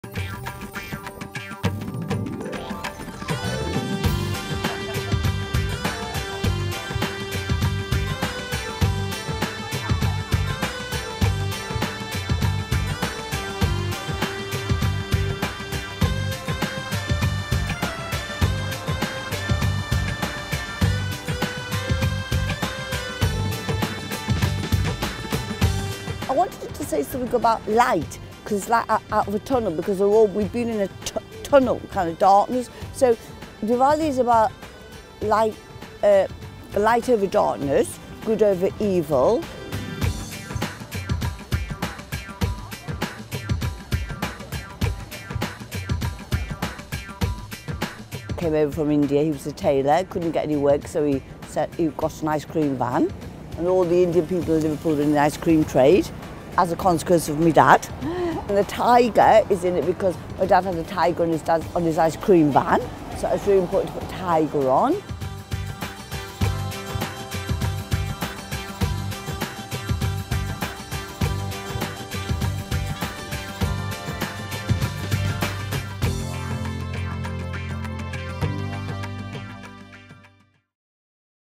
Glittering Tate Britain light show celebrates Diwali
😷 Got the lockdown blues? ☔️

✨ Tate Britain is bringing some joy to London’s gloomy skies with a stunning neon artwork celebrating Diwali, the Indian festival of lights ✨